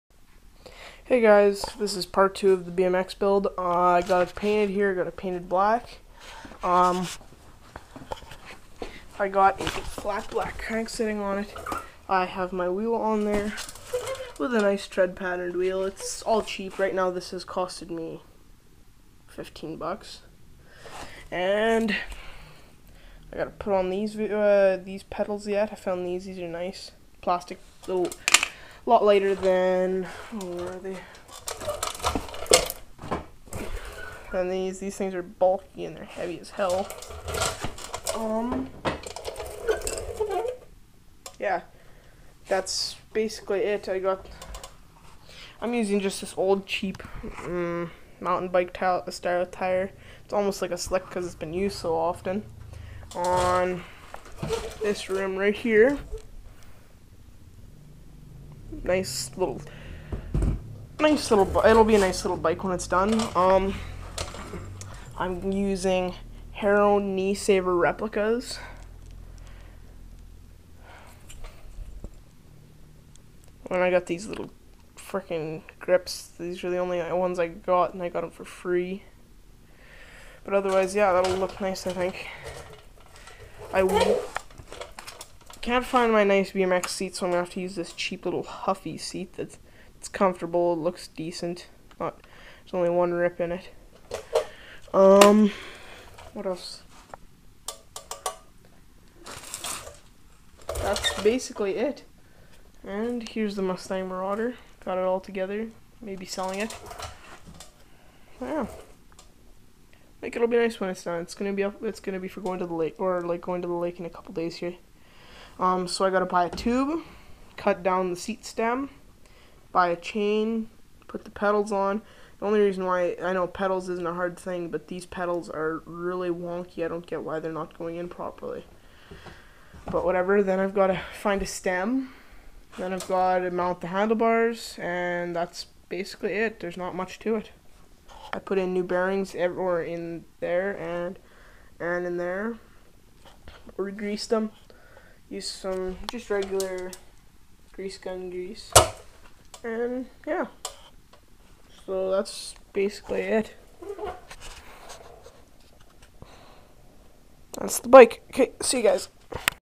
BMX Project Part 2
This is the second part of my updates on my BMX projects. All thats left to do is put the pedals on, buy a chain, get pegs, put on the seat, get a stem, and mount the handle bars.